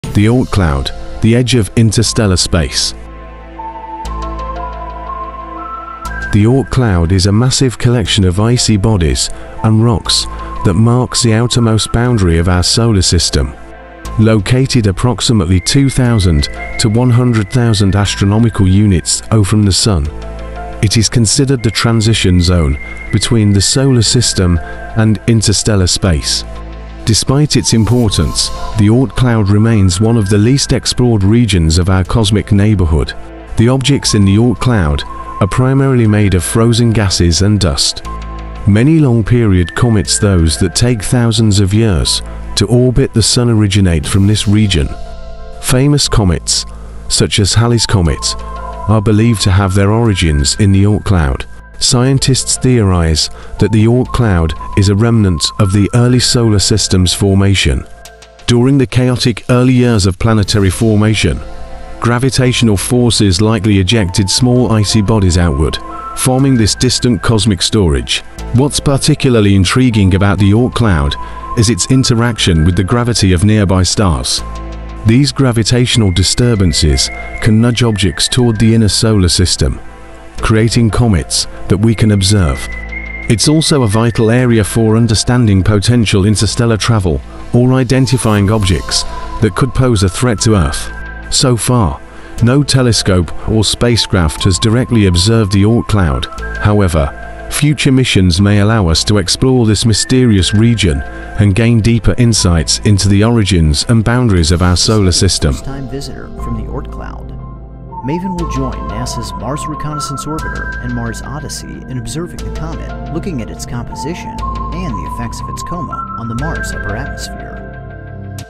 The Oort Cloud: The Edge of Interstellar Space
"The Oort Cloud is a mysterious region marking the outermost edge of our Solar System. Located up to 100,000 astronomical units away, it is home to icy bodies and the birthplace of long-period comets. Scientists believe it holds clues to the Solar System’s formation and may one day reveal secrets about interstellar travel. What mysteries does this cosmic boundary hold? Explore the edge of the Solar System with us!"